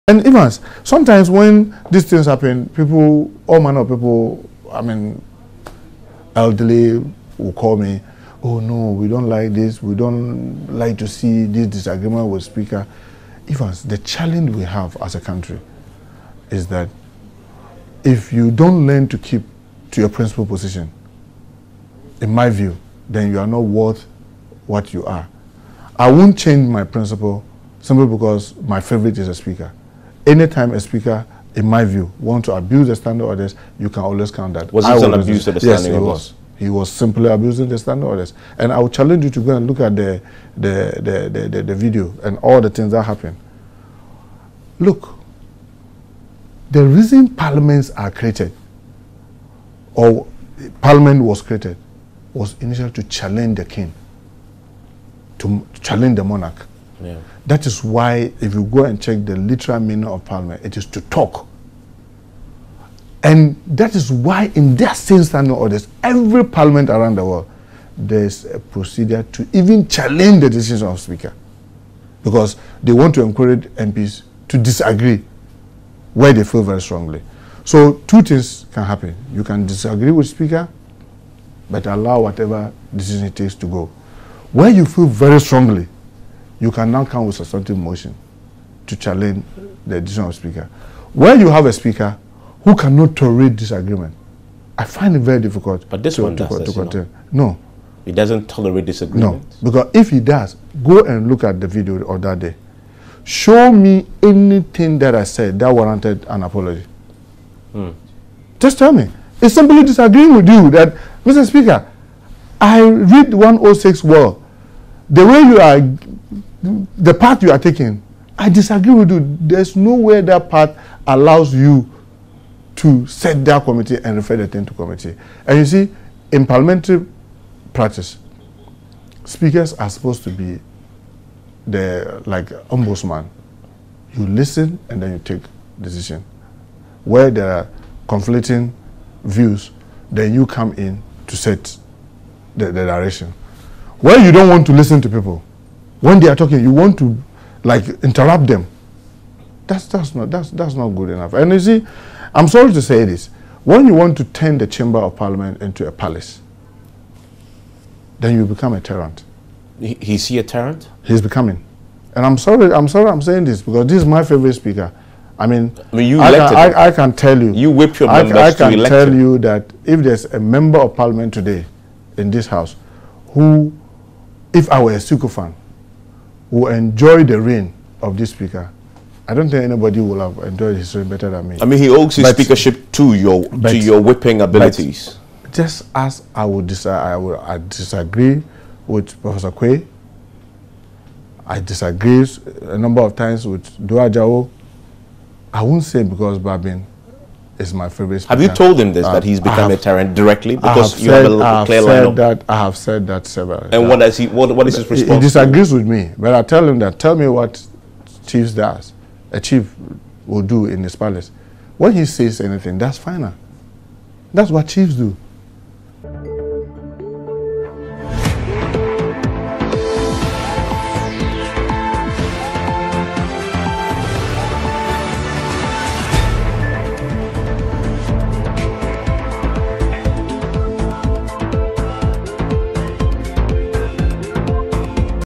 Alban Bagbin becoming a tyrant; he messed up Ken Ofori-Atta's censure hearing - Muntaka Mubarak
Alban Bagbin becoming a tyrant; he messed up Ken Ofori-Atta's censure hearing - Minority Chief Whip, Muntaka Mubarak.